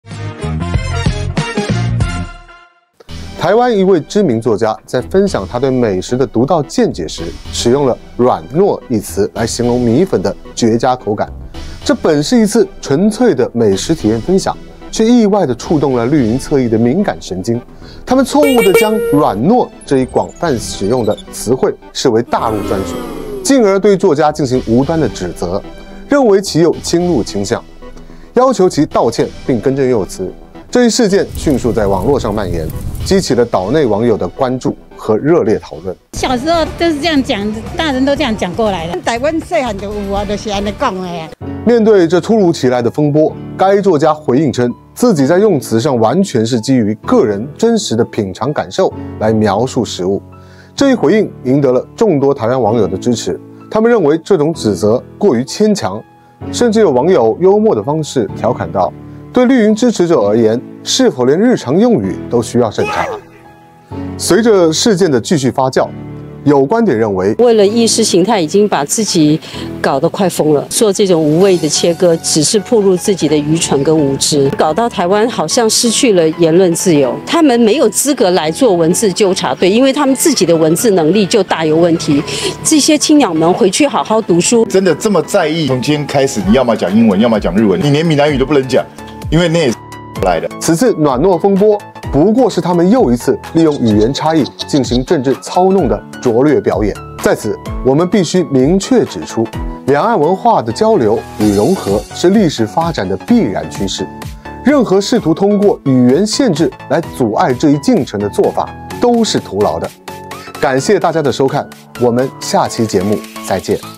蝦侃台 【台灣搞「文字獄」？說「軟糯」也被網暴，綠營側翼挨轟：回去讀書】
【台灣搞「文字獄」？說「軟糯」也被網暴，綠營側翼挨轟：回去讀書】